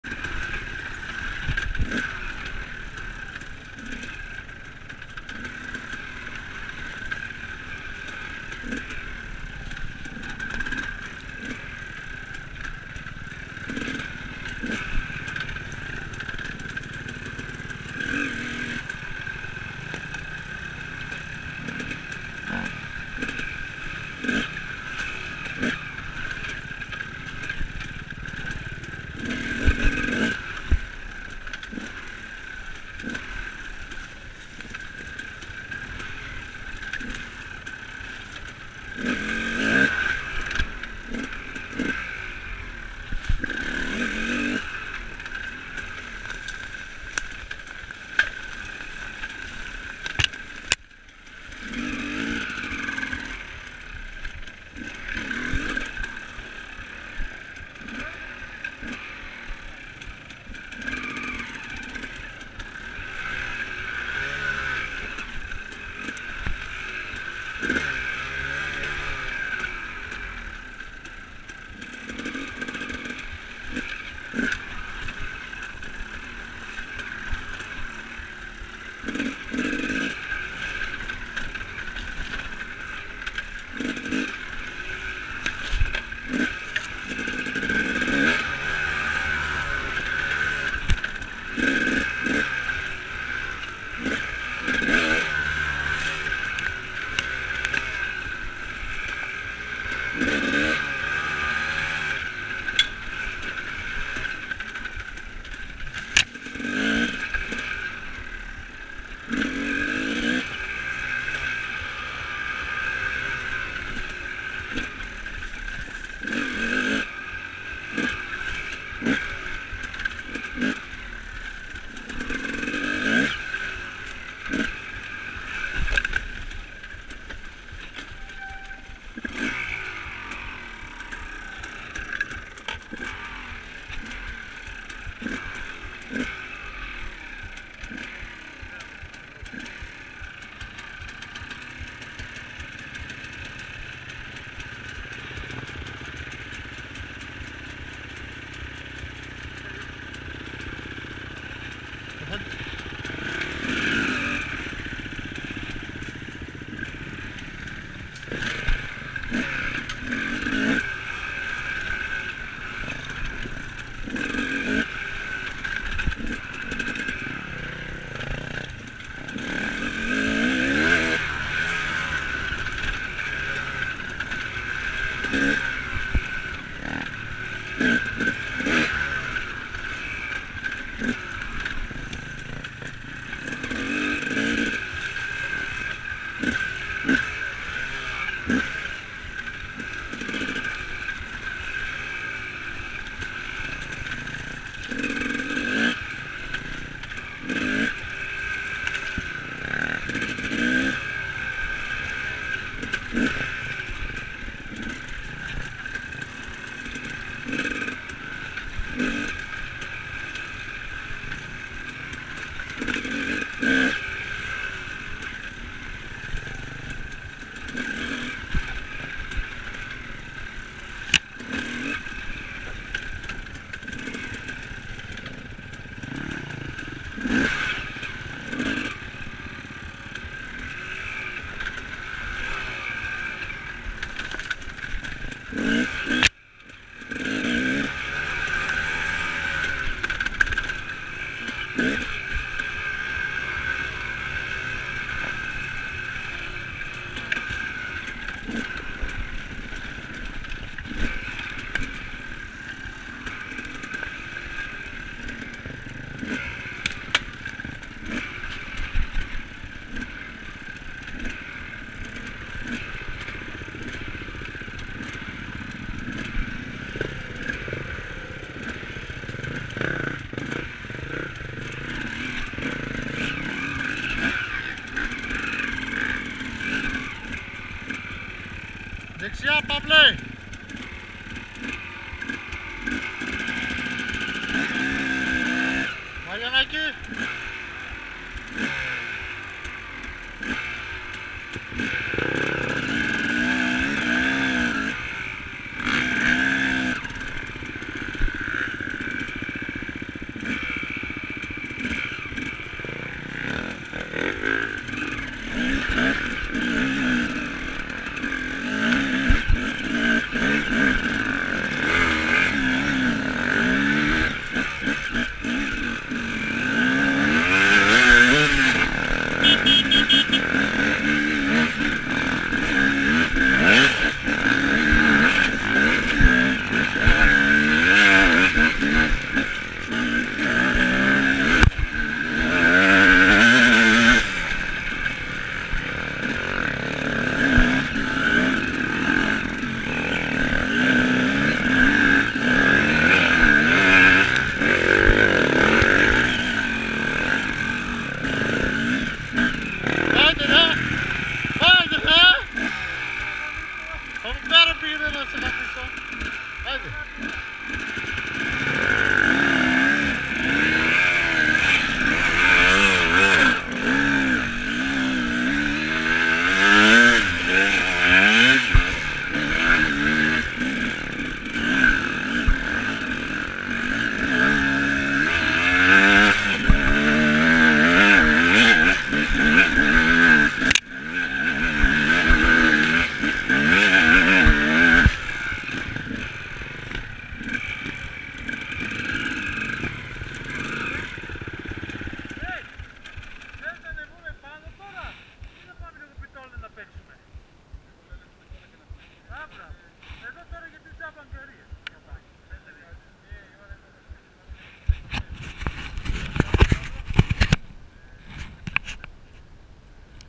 28/6/20 κατάβαση (παυλίνι) enduro team florina with ΜΟΑ
βολτούλα στα βουνά γύρο απο τον πολυποταμο Φλώρινας με φίλους επισκέπτες απο το μοτοσυκλετιστικο όμιλο Αμύνταιου (ΜΟΑ)